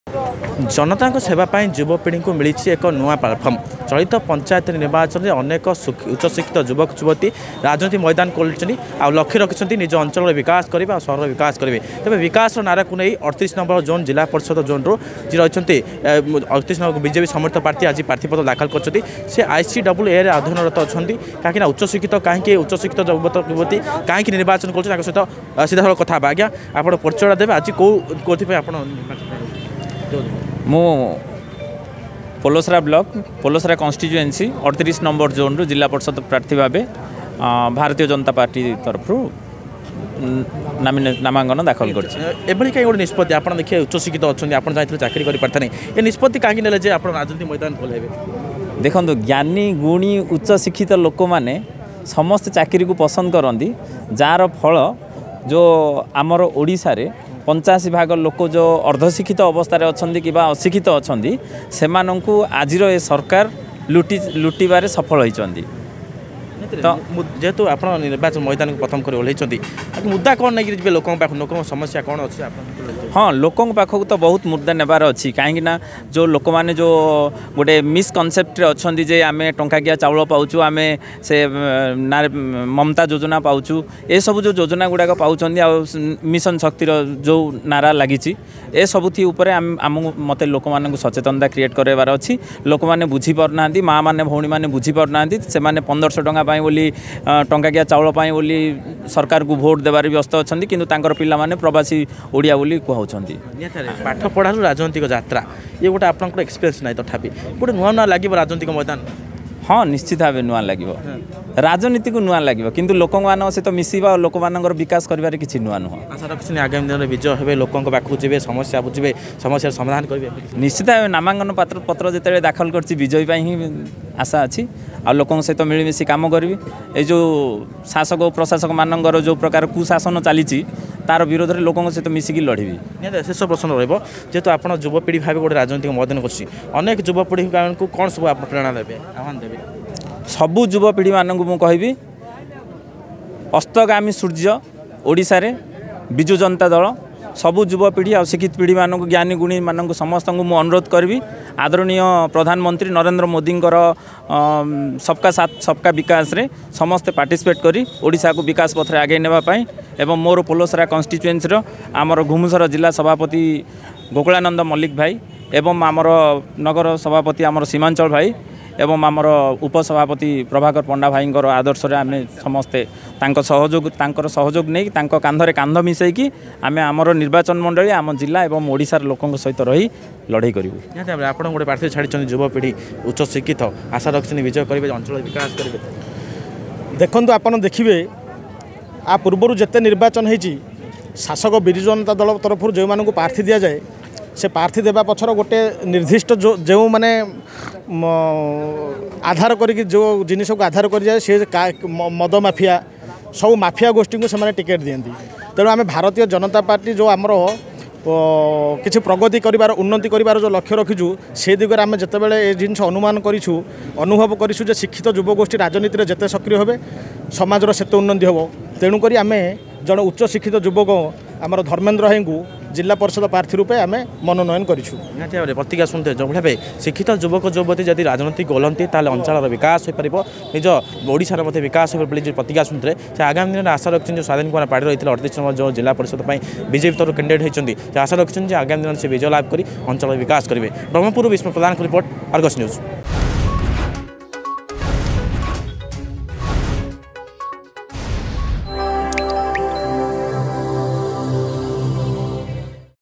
ରାଜନୀତି ମଇଦାନରେ ଉଚ୍ଚ ଶିକ୍ଷିତ ପ୍ରାର୍ଥୀ ଧର୍ମେନ୍ଦ୍ର
ଚଳିତ ପଞ୍ଚାୟତ ନିର୍ବାଚନରେ ଉଚ୍ଚ ଶିକ୍ଷିତ ପ୍ରାର୍ଥୀଙ୍କ ଚେହେରା ସାମ୍ନାକୁ ଆସୁଛି । ସ୍ୱାଧୀନ ଆଇ ସି ଡ଼ବ୍ଲ୍ୟୁ ଏ କୋର୍ସ କରି ଉଚ୍ଚଶିକ୍ଷିତ ଥିବା ବେଳେ ପ୍ରଧାନମନ୍ତ୍ରୀଙ୍କ ବିକାଶ ମଧ୍ୟରେ ଗଞ୍ଜାମ ଜିଲ୍ଲା ପୋଲସରା ନିର୍ବାଚନ ମଣ୍ଡଳୀ ୩୮ ନଂ ଜୋନରେ ବିଜେପି ସମର୍ଥିତ ପ୍ରାର୍ଥୀ ଧର୍ମେନ୍ଦ୍ର କୁମାର ପାଢୀ ଜିଲ୍ଲାପରିଷଦ ପ୍ରାର୍ଥୀ ରୂପେ ନମାନଙ୍କ ପତ୍ର ଦାଖଲ କରିଛନ୍ତି ।